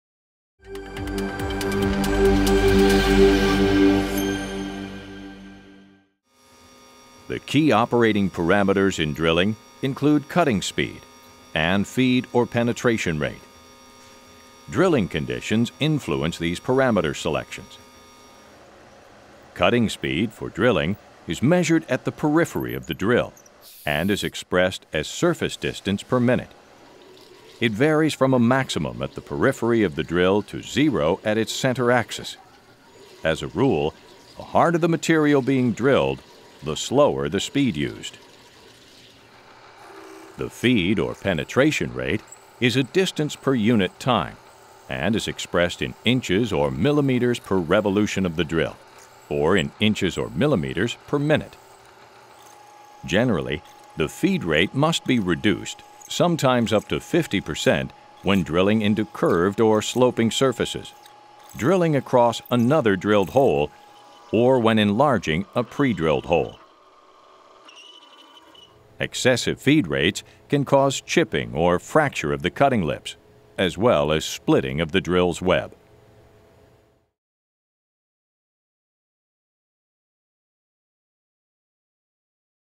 Holemaking
Holemaking is the most common machining operation, consuming half of all cutting tools used in chip making operations. Part of the Fundamental Manufacturing Processes Video Series, this program examines drilling basics, the drill, holemaking machines, drilling operating parameters, and hole finishing operations.

The drilling basics segment details chip generation, high pressure coolant systems, short hole drilling, peck drilling, deep hole drilling, trepanning, EDM holemaking, and laser holemaking.

The drill segment takes an extensive look at the elements that make-up the twist drill, such as the shank, flutes, drill point, cutting lips, chisel edge, and drill web. The various drill types are also featured, including: indexable insert drills, center drills, spade drills, core drills, step drills, drilling end mills, and drill jigs.

The holemaking machines segment explores the sensitive drill press, radial drills, self-feeding drill units, multi-spindle drilling machines, and drill and tap centers.

The drilling operating parameters segment presents the variables of cutting speed, feed/penetration rate, drill bluntness, and drill point grinding.

The hole finishing operations segment covers reaming, boring, honing, roller burnishing, counterboring, and countersinking.